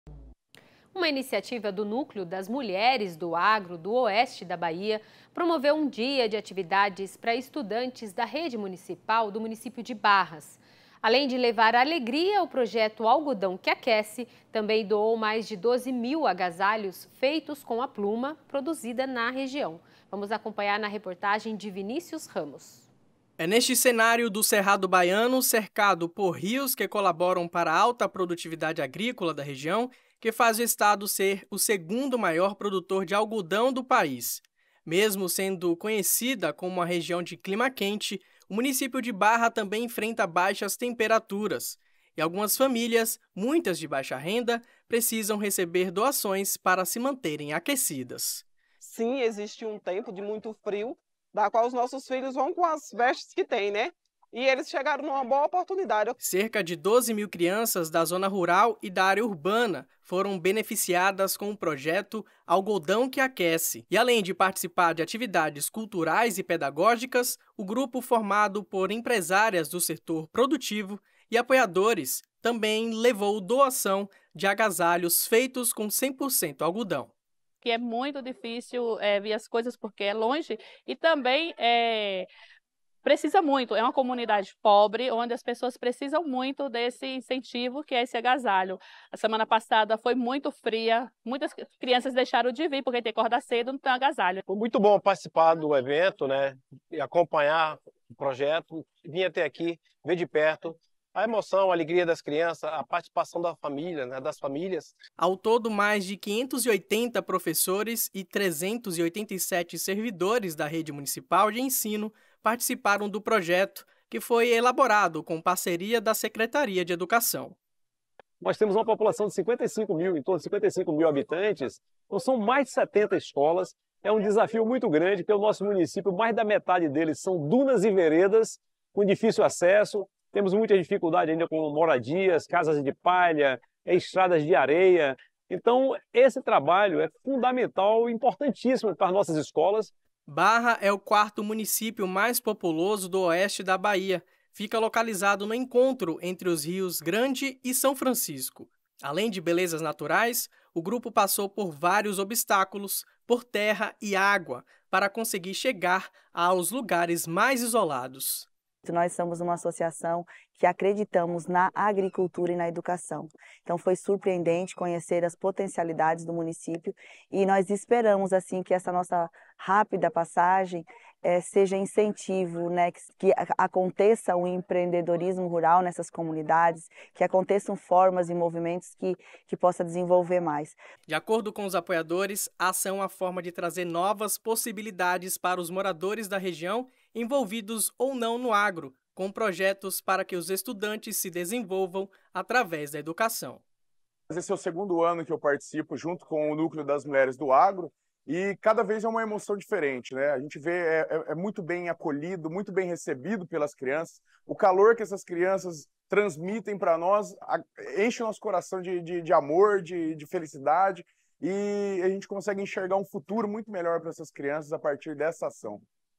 Projeto doa casacos para crianças e ensina sobre a cultura do algodão | Canal Rural
Iniciativa é liderada pelo Núcleo Mulheres do Agro, beneficia alunos do oeste da Bahia e conta com apoio de empresas e entidades do setor.